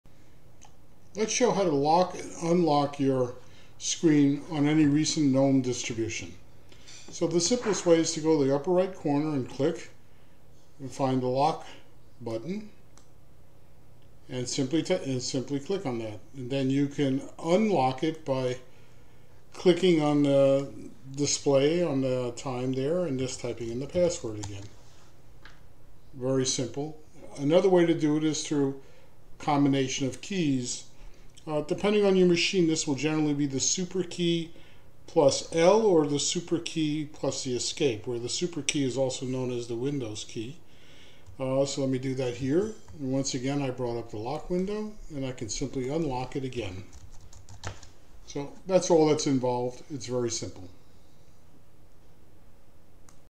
Locking and Unlocking the Screen
This video is about how to lock and unlock your screen on any recent GNOME distribution.